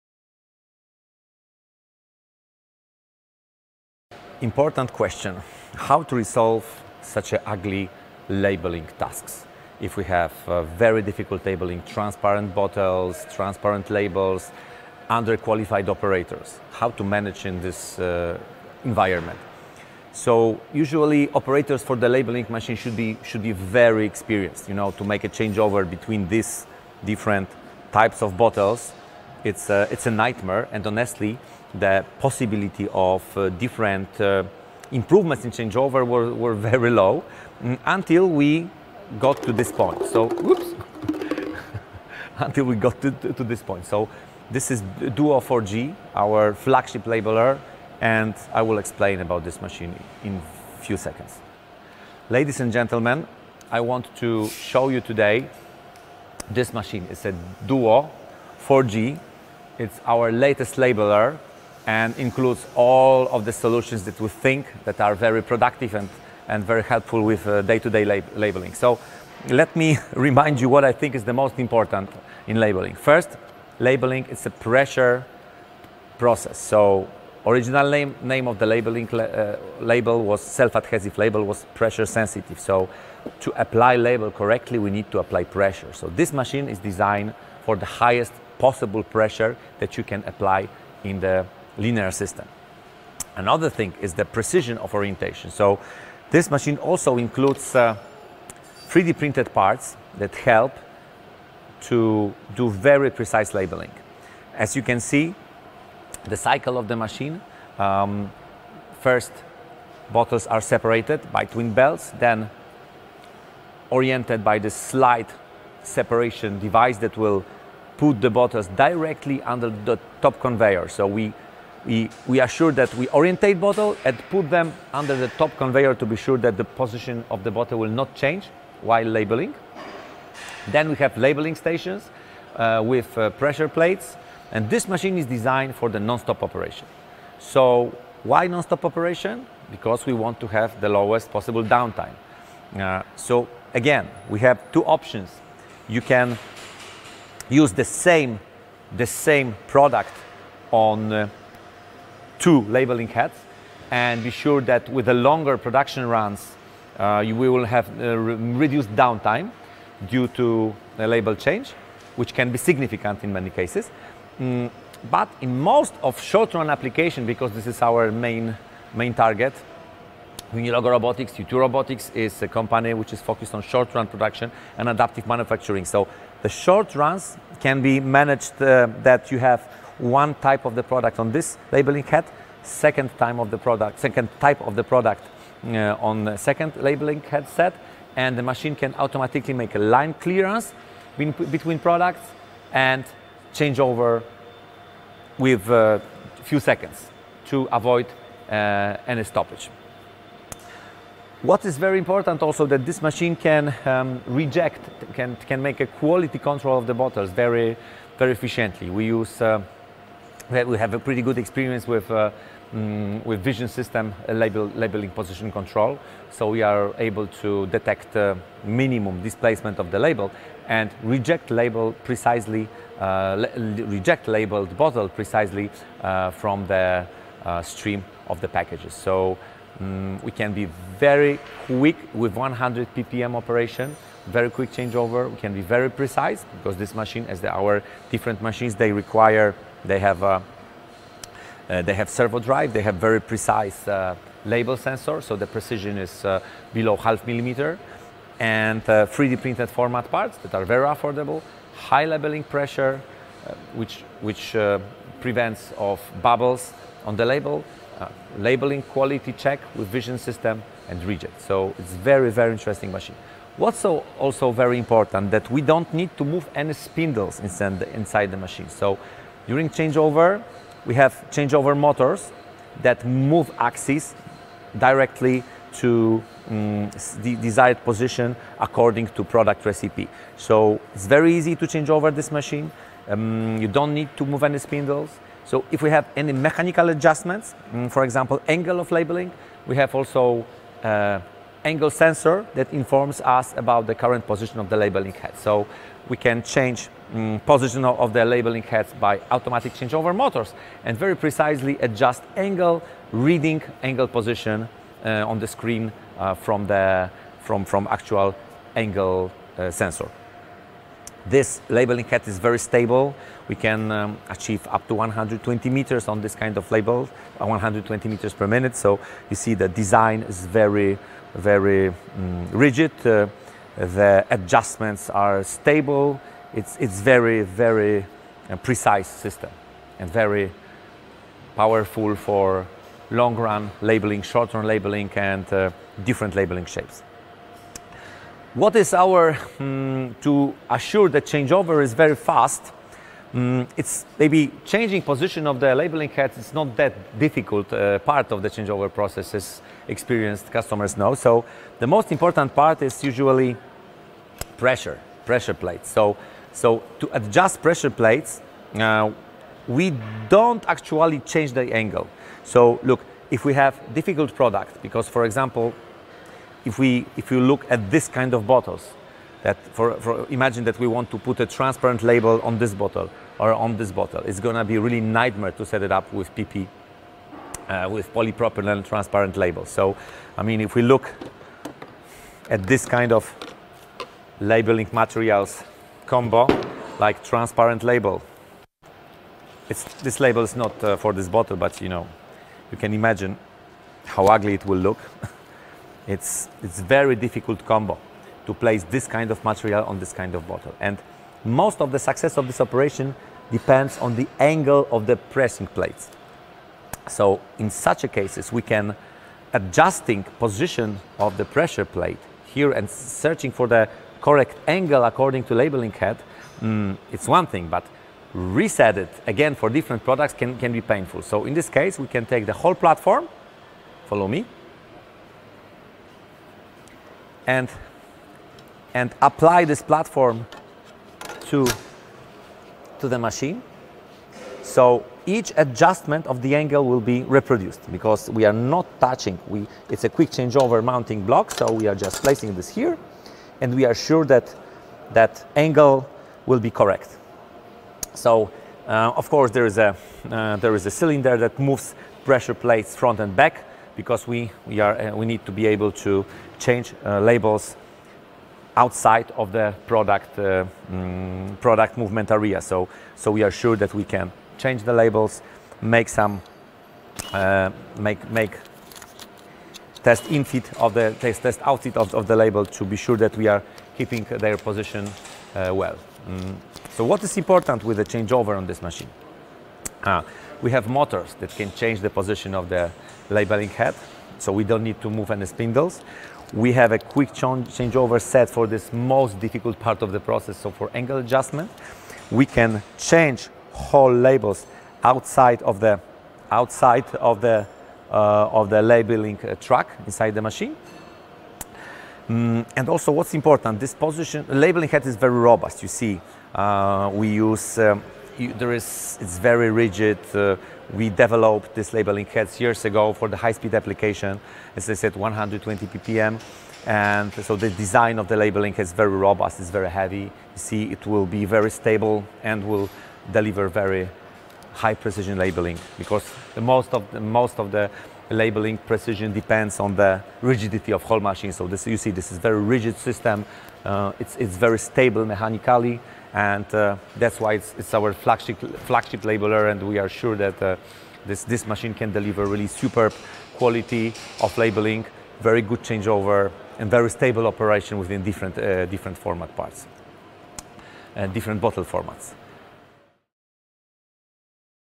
Labelling machine DUO 4G
Looking for a step-by-step guide to labelling? Or maybe you’re wondering how the latest technologies allow some companies to always stay 2 steps ahead of their competition?

Well, we have just the video for you. Quick changeover, 3D printed format parts and pinpoint precision are common features of our machines, but today we want to give the spotlight to our labelling machine DUO 4G.

Take a tour of the labelling machine and discover the most important elements of labelling and why our clients are able to stay on top of their game in the dynamically changing markets.